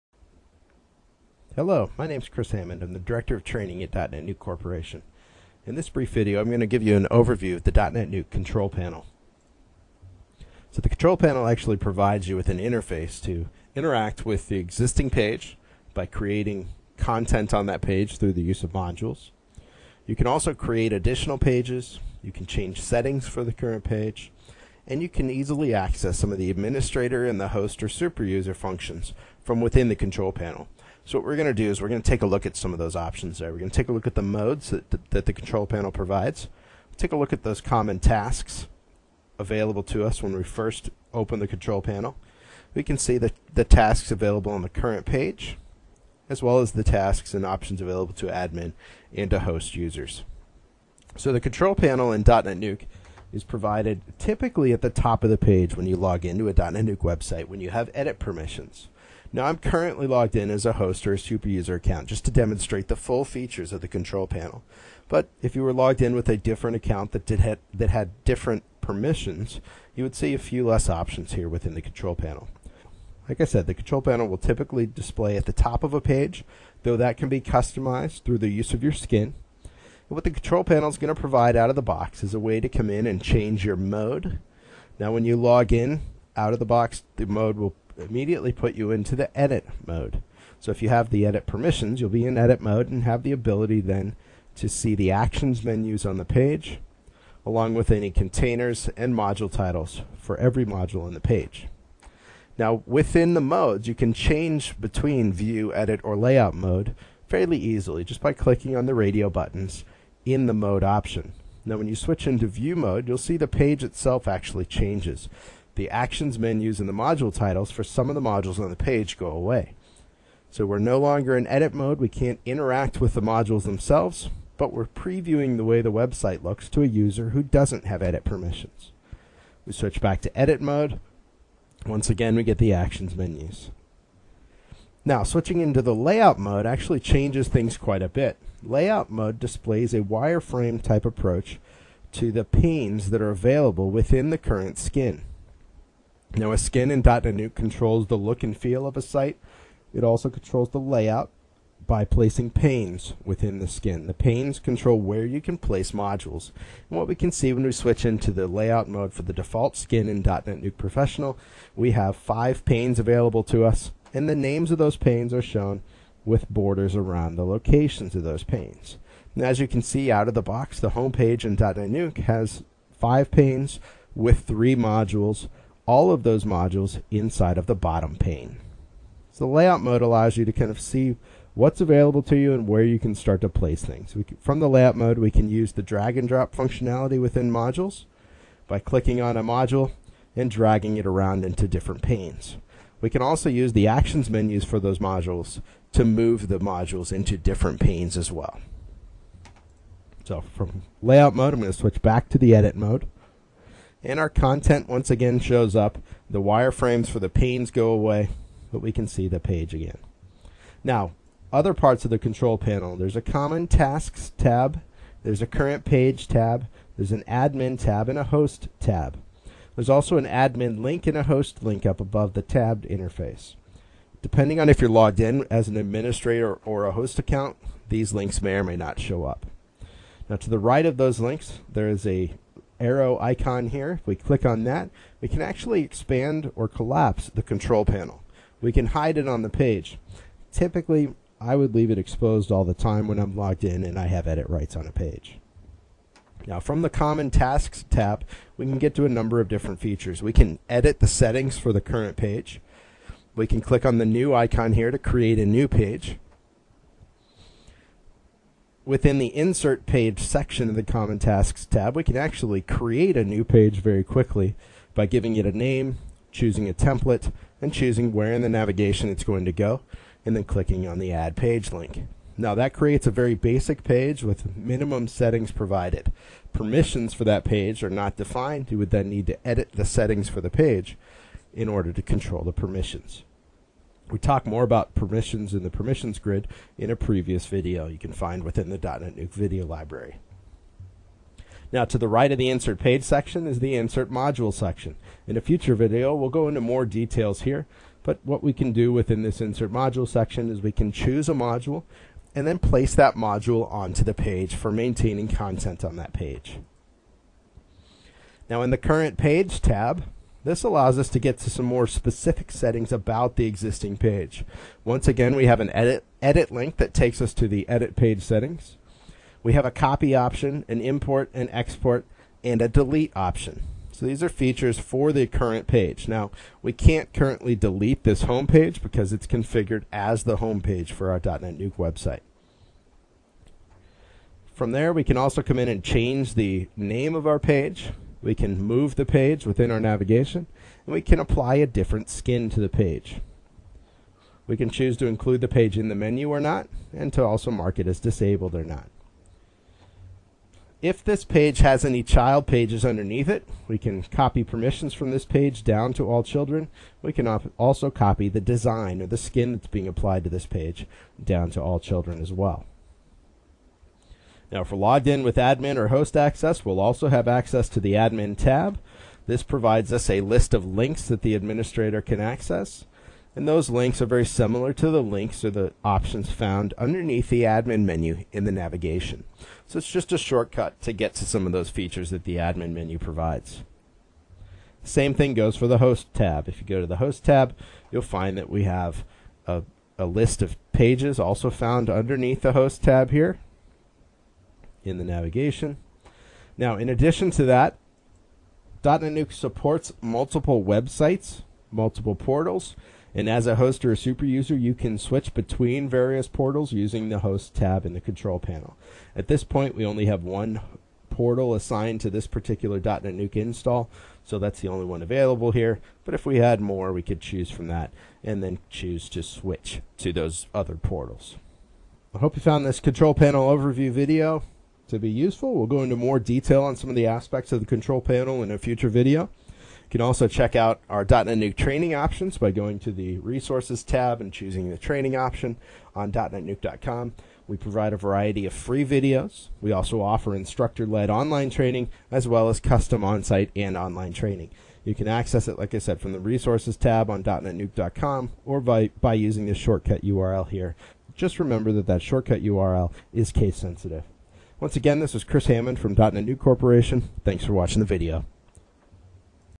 DotNetNuke Control Panel Overview
In this video Chris Hammond provides you an overview of the DotNetNuke Ribbon Bar Control Panel using DotNetNuke Professional 5.6.0. This also applies to DNN Community Edition starting with version 5.4.0 and up.
Category: Administration
Author: Chris Hammond